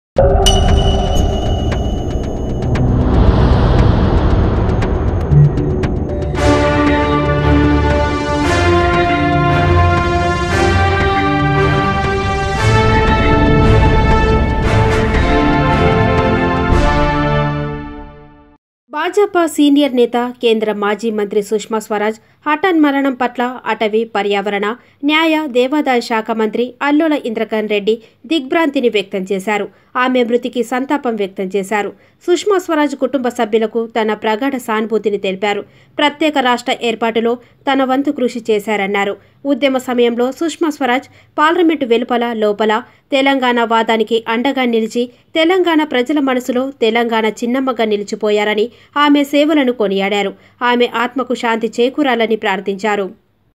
బీజేపీ సీనియర్ లీడర్ సుష్మా స్వరాజ్ మృతి సంతాపం తెలిపిన మంత్రి అల్లోల
BJP Senior Leader Sushma Swaraj Pass Away... Minister Indrakaran Reddy Santhapm